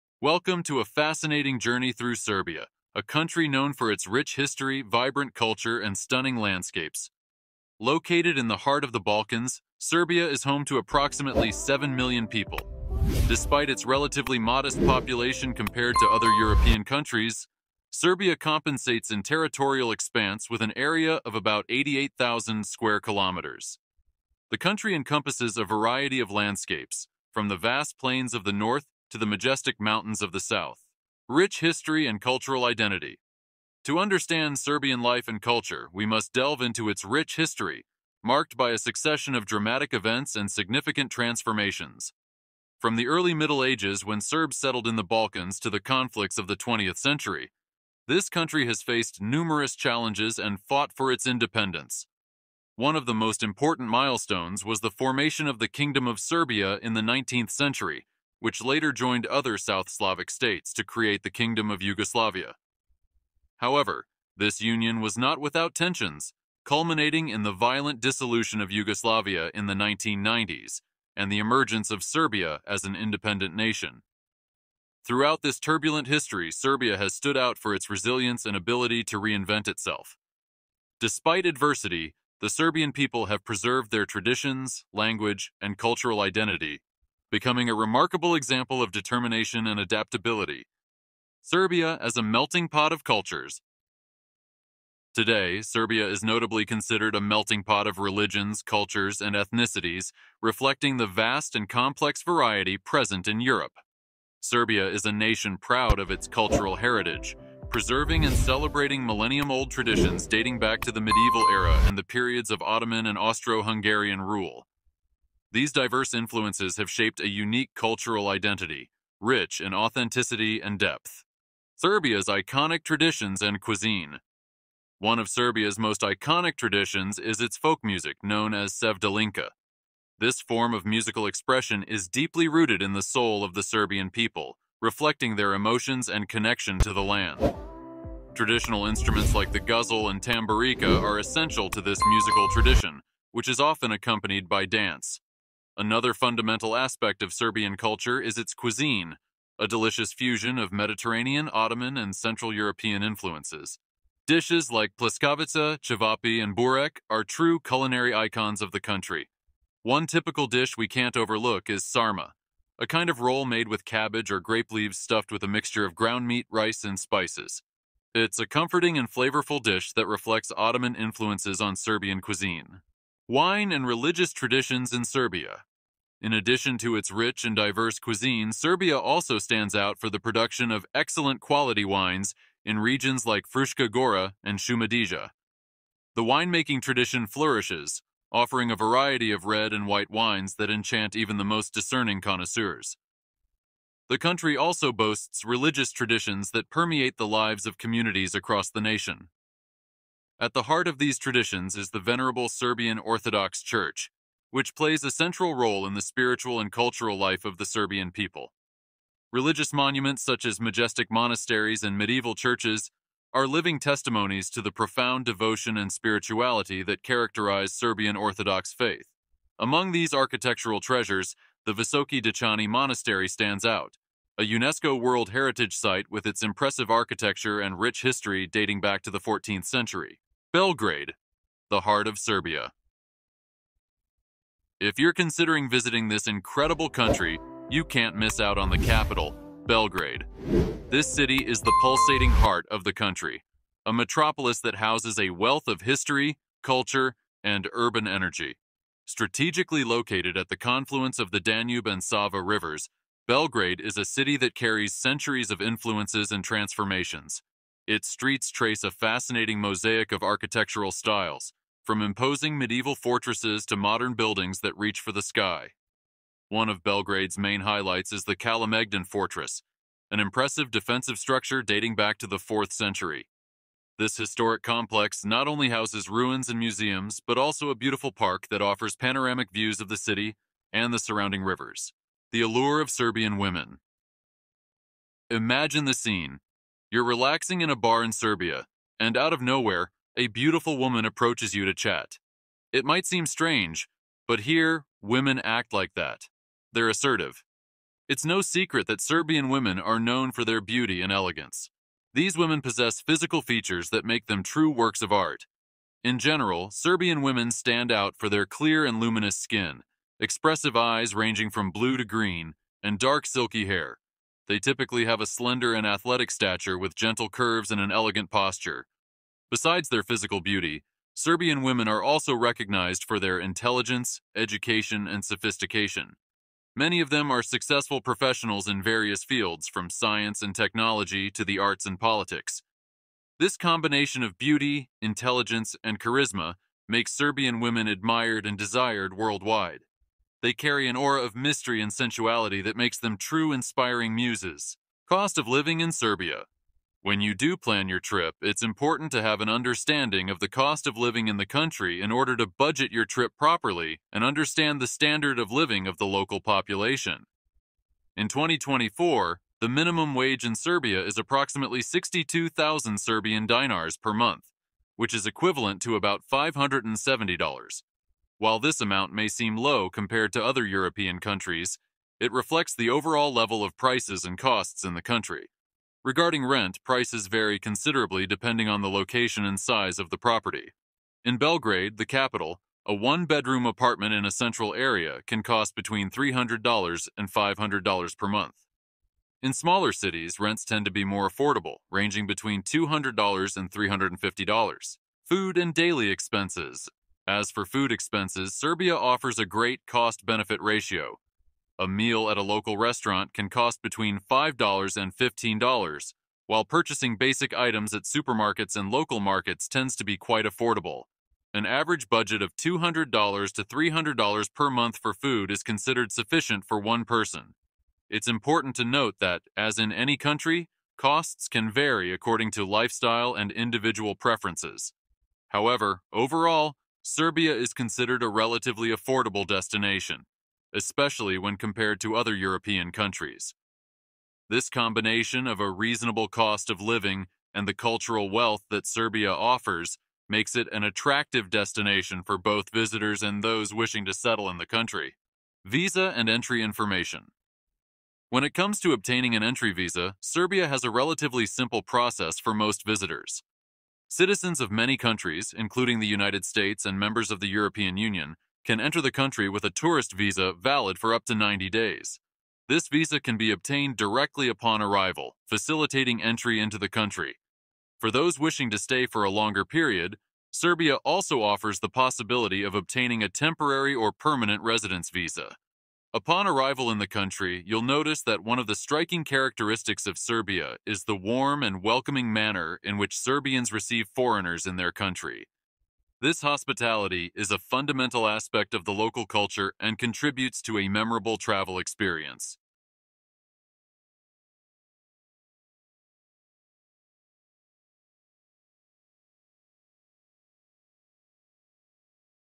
A Glimpse into the Deprived Country with Unconventional Women
This video is about Serbia and its unique women, who impress with their boldness and beauty. I'll talk about life in this country, its culture and traditions, as well as what makes Serbian women so special. Join in to learn more about Serbia!